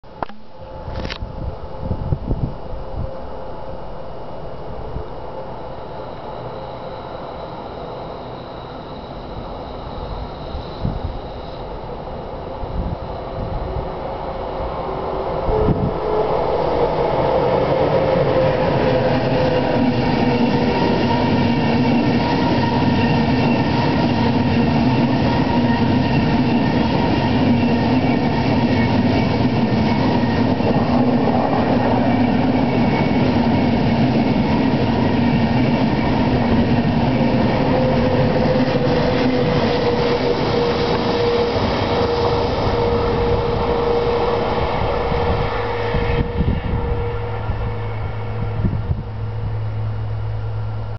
ICE 1 DB
Tren ICE 1 en dirección a Stuttgart bajando la pendiente de Geislingen el 6 de febrero de 2008.